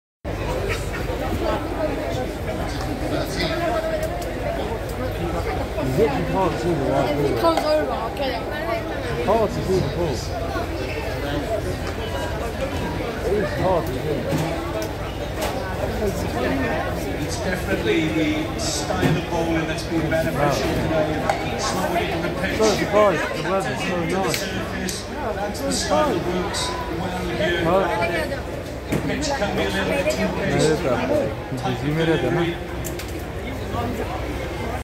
Daryl Mitchell practicing on Lord's cricket ground for
In this video, I'm thrilled to share my once-in-a-lifetime experience at the iconic Lord's Cricket Ground, the home of cricket. Join me as I take you behind the scenes of the Hundred Tournament, where I had the incredible opportunity to watch international cricket stars in action and even meet some of my cricketing heroes up close!

From the electric atmosphere in the stands to the breathtaking performances on the pitch, this day at Lord's was nothing short of spectacular. I'll walk you through the thrilling moments, the crowd's energy, and the heart-pounding matches that unfolded before our eyes.

But that's not all! Stick around as I share the unforgettable moments when I had the privilege of meeting international cricket legends and getting insights into their experiences and strategies. It was a dream come true for any cricket enthusiast, and I can't wait to relive it with you.

Whether you're a die-hard cricket fan or just curious about the excitement of live cricket matches, this video will transport you to the heart of the action at Lord's Cricket Ground.